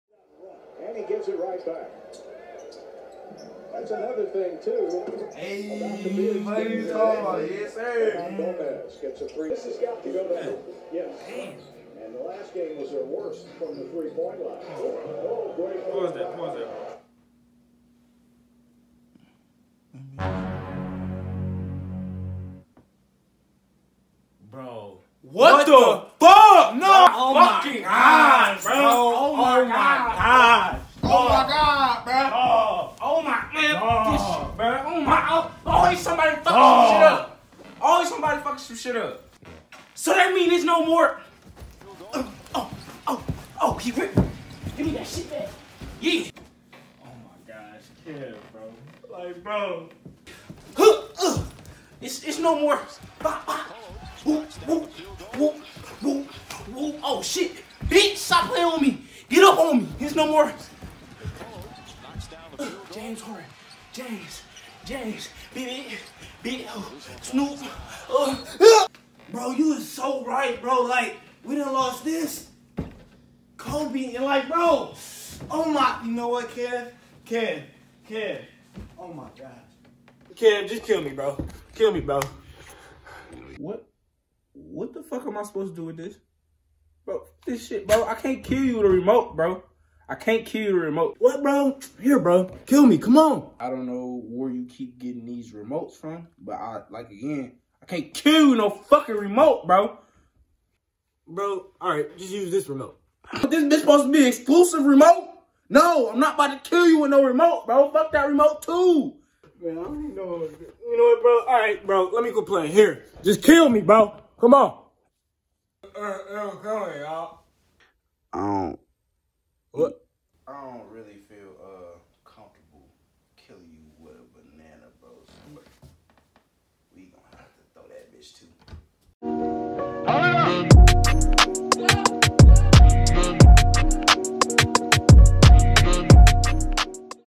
When The NBA Season Got Suspended
This is a little skit... let me know if you like it, if you want more of these show love in the comment section